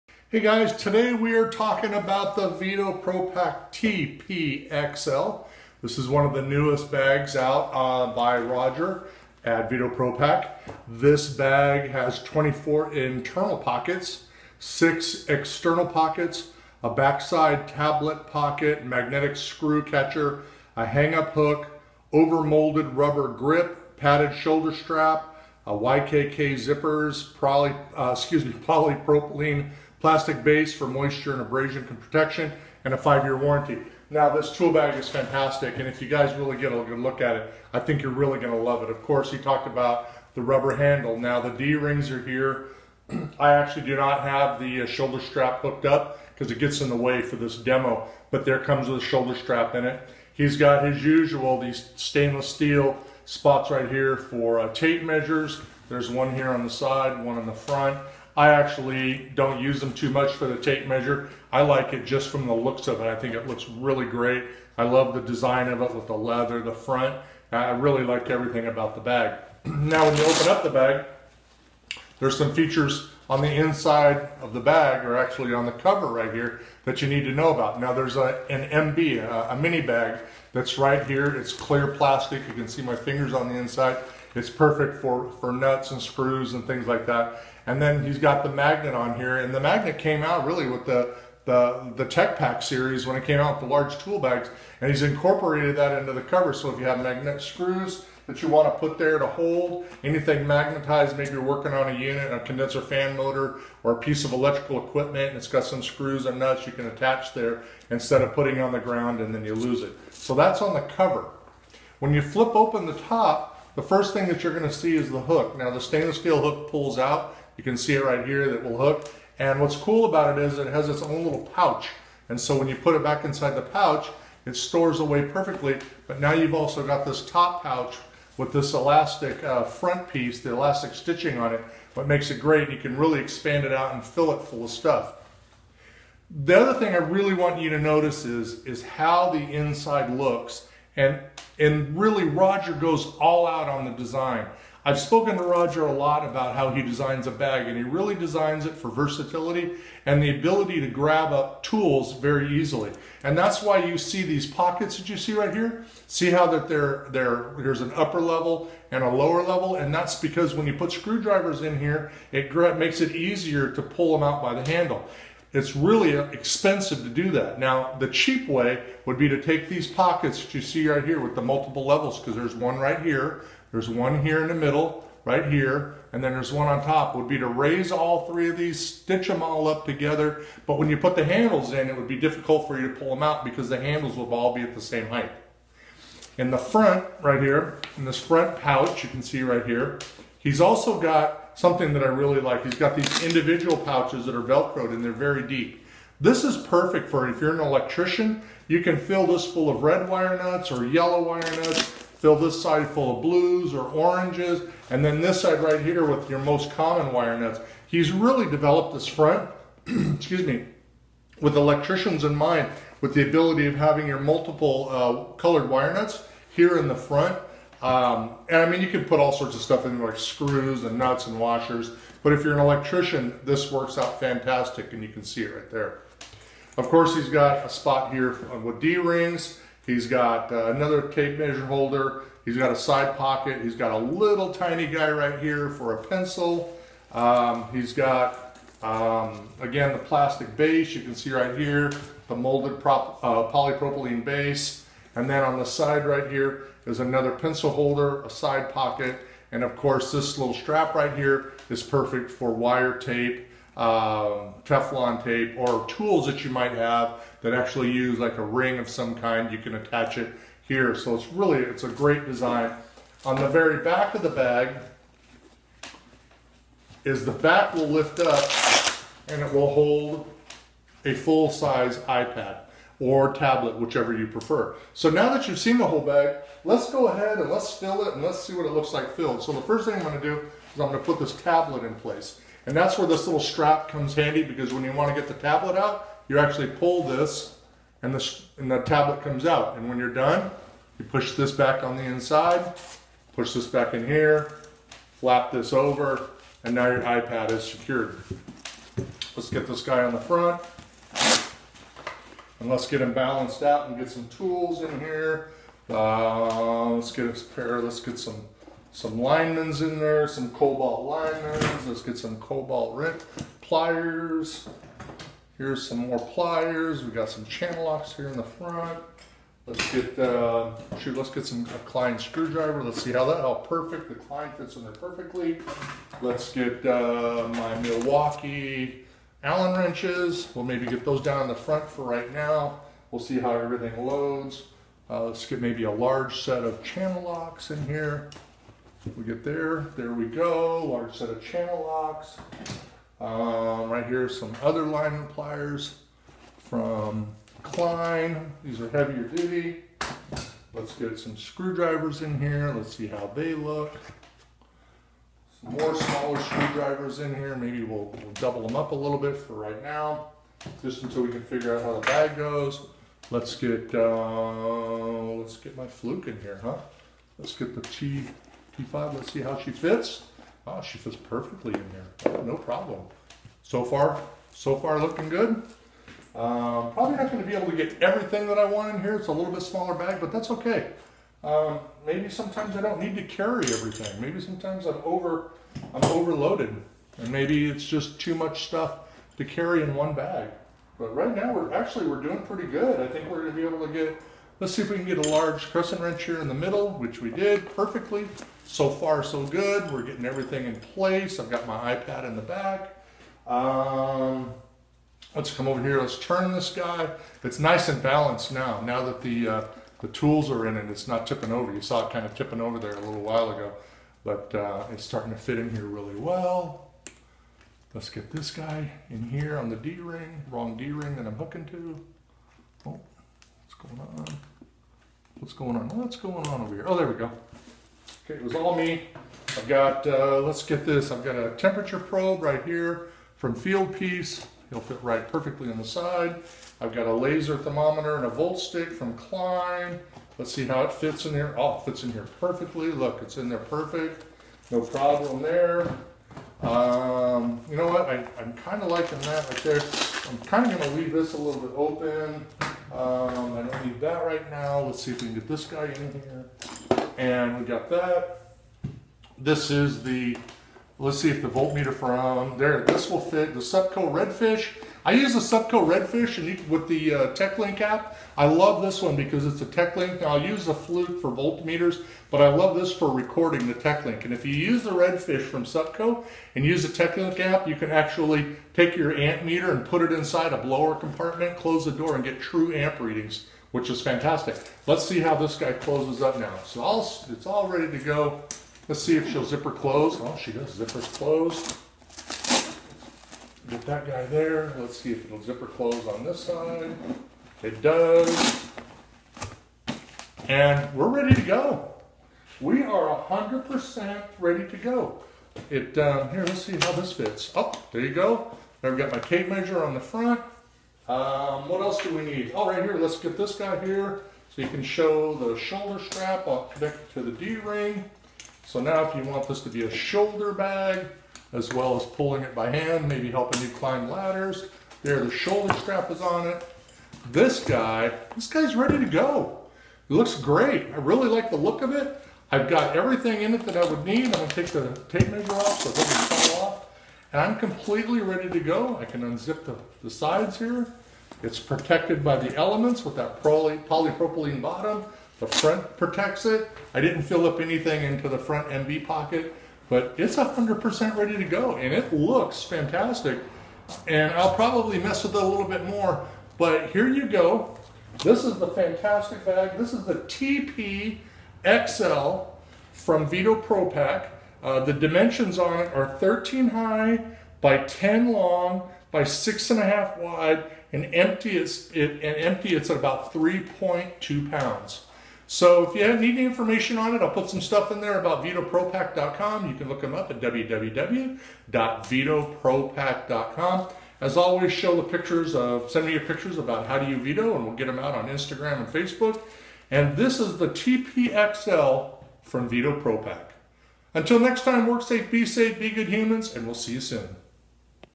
Tool Bag Review on the Veto TP-XL
Tool Bag Review about the TP-XL from Veto Pro Pac. This is an awesome bag with some amazing features. Lots of pockets and a IPAD pocket and retractable hook. You're gonna love this bag.